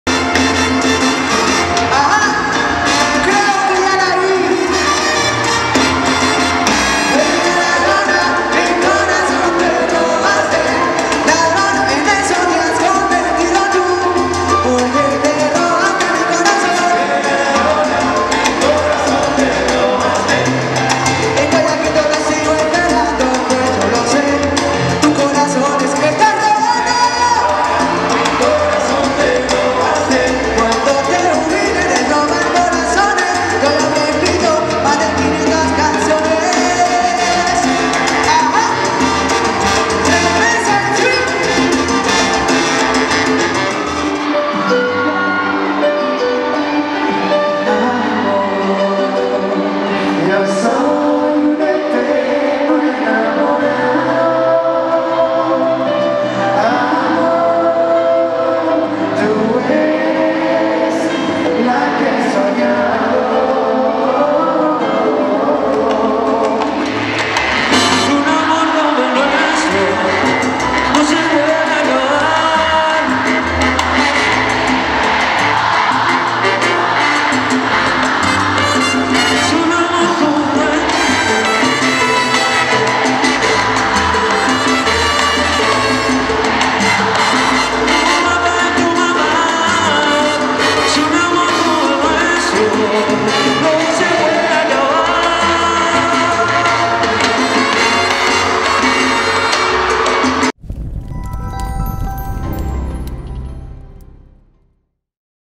Salserín emociona a sus fans en Guayaquil
El grupo de salsa brindó un concierto en el centro de convenciones Simón Bolívar. Video: Ronald Cedeño